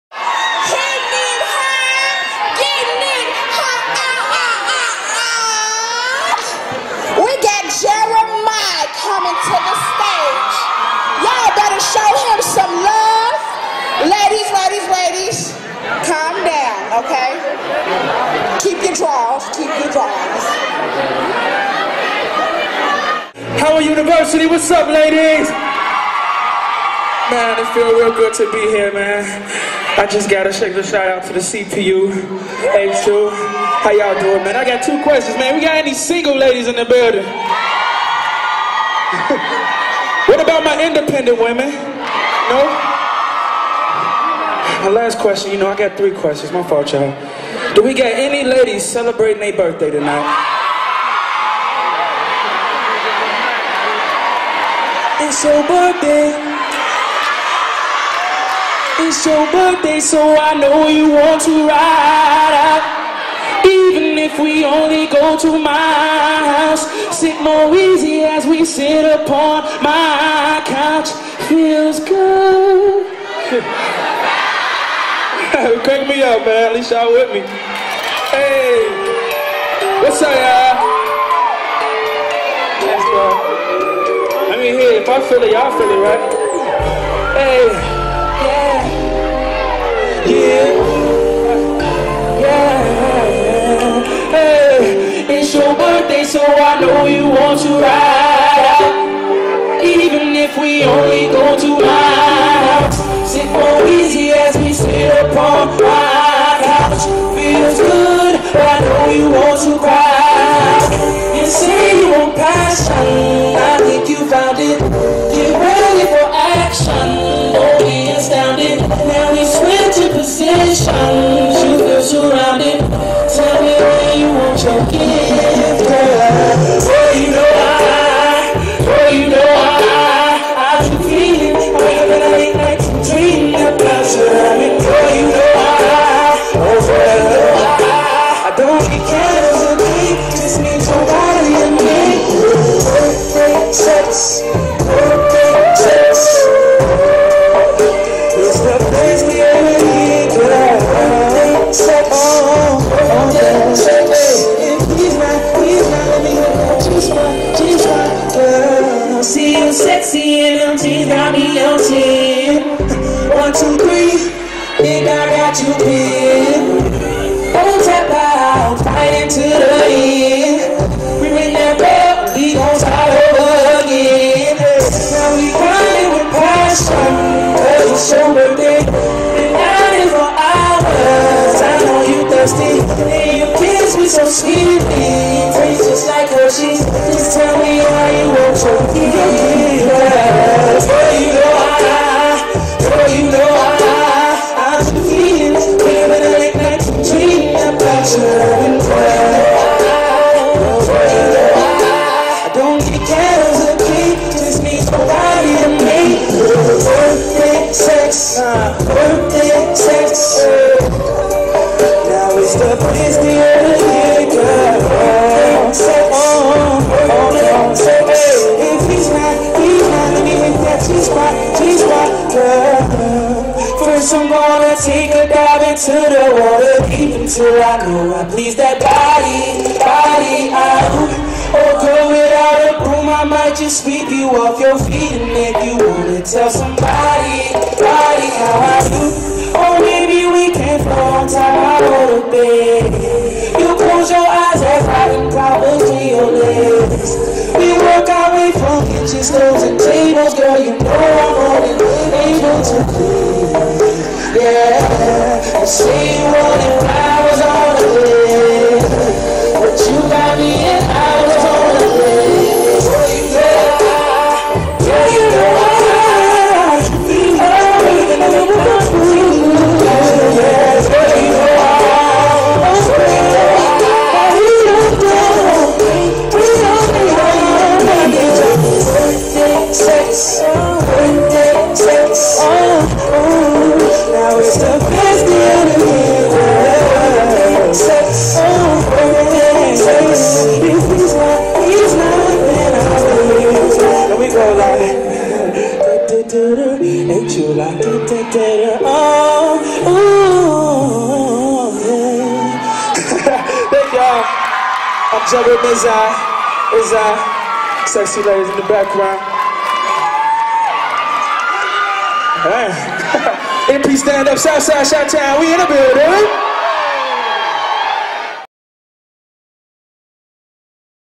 Jeremih at Howard University in DC
Jeremih performing at Howard University in Washington DC.